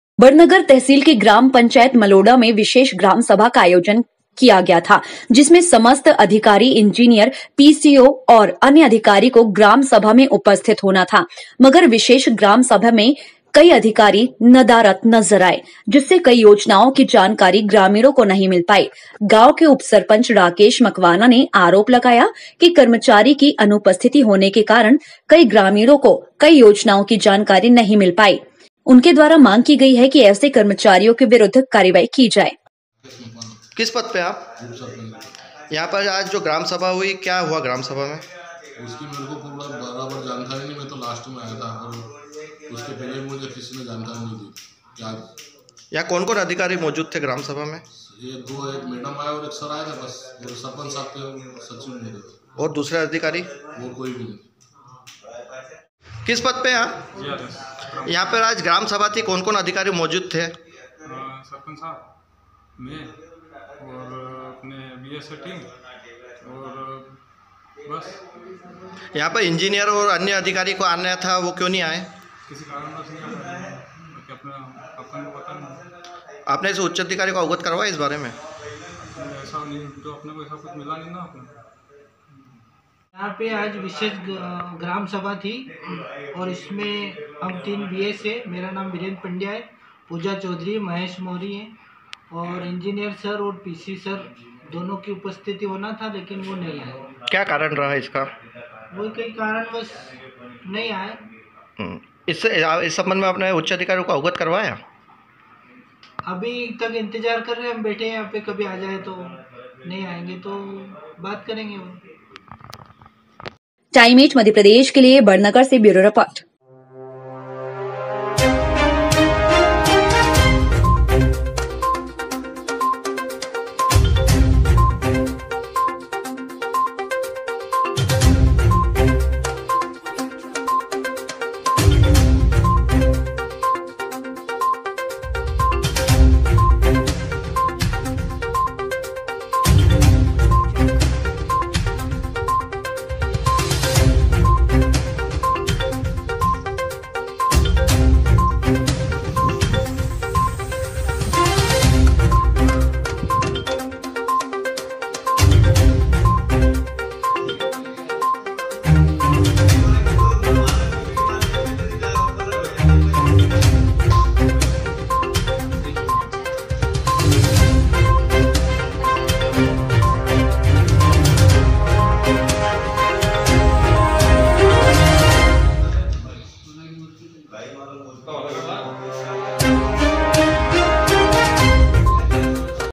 ग्रामसभा से बेरुखी । TIME8 MADHYA PRADESH
उज्जैन। बड़नगर तहसील के ग्राम पंचायत मलोडा मैं विशेष ग्राम सभा का आयोजन  सभा का आयोजन किया गया था।